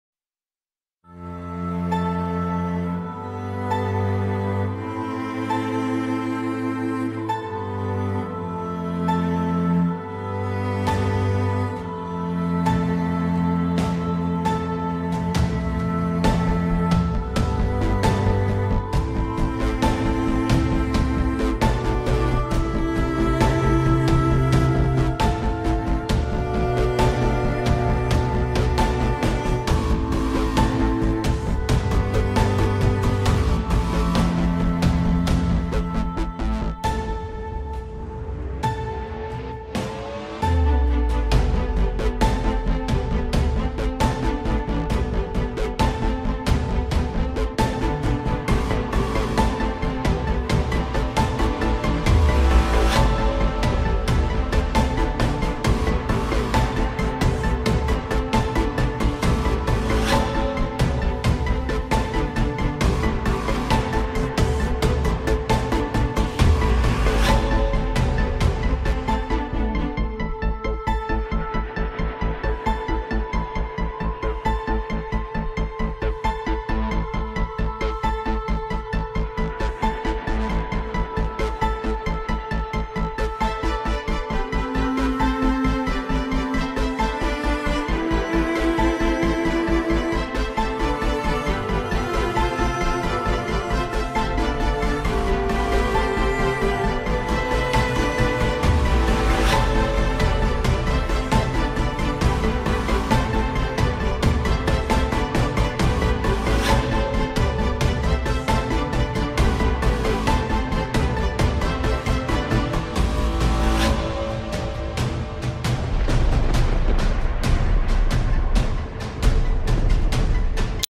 Sanford Flooding (Main Street Both Directions) Dam Failure May 23, 2020!
Many lost everything and MOST did not have FLOOD INSURANCE!   BUT MULTIPLE DAMS FAILED CAUSING DESTRUCTION OF SANFORD.